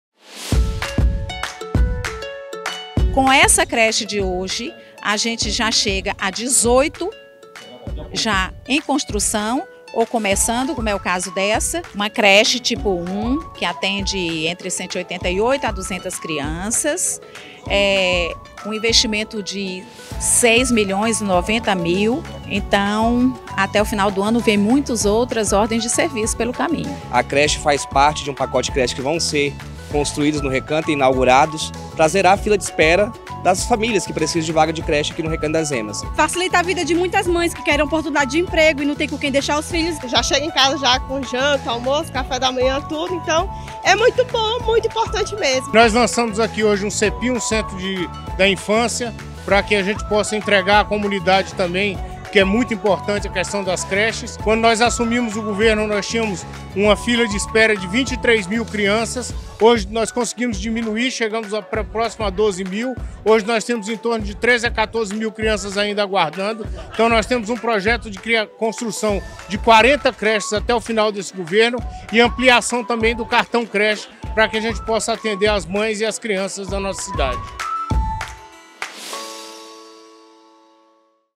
Recanto das Emas terá creche em tempo integral para 188 alunos
GDF investe R$ 6 milhões na unidade e entregará 40 Centros de Educação Primeira Infância (CEPIs) até 2026, ampliando a oferta no ensino infantil.

Fique por dentro de tudo o que acontece no DF.
Acesse o site da Agência Brasília ou baixe o aplicativo Agência Brasília Mobile